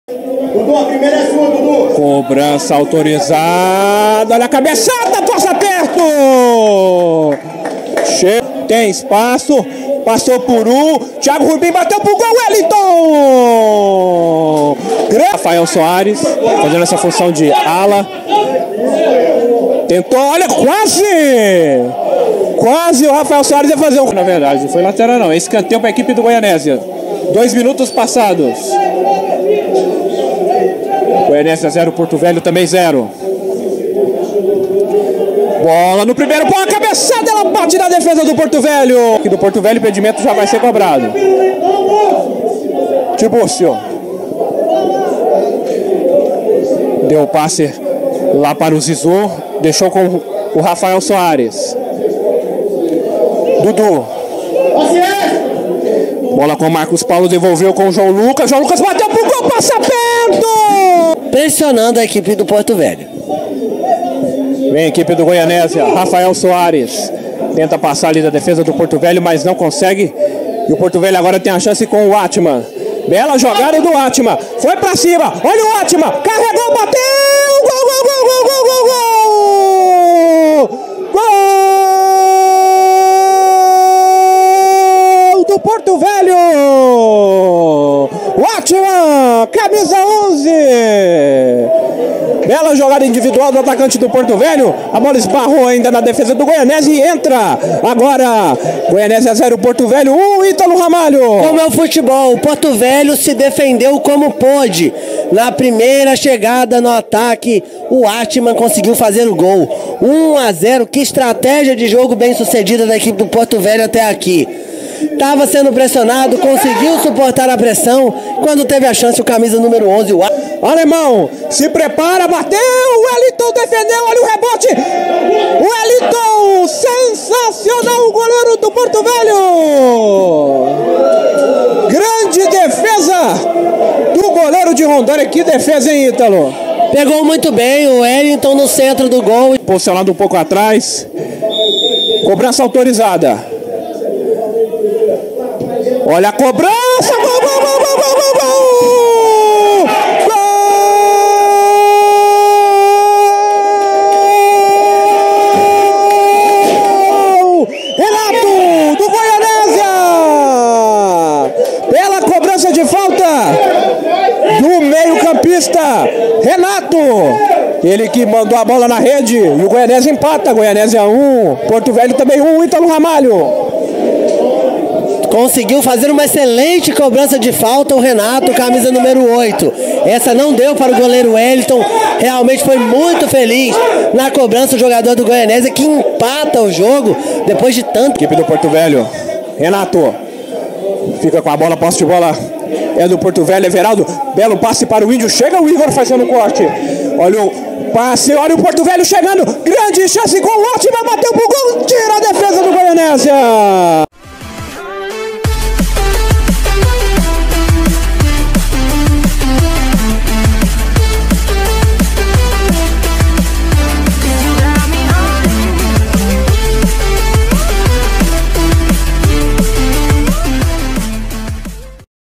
GOIANESIA 1 X 1 PORTO VELHO | BRASILEIRAO SERIE D | 14/08/21
Sejam bem-vindos!!

Goianesia empata em casa por 1x1 com o Porto Velho pelo brasileirao serie D.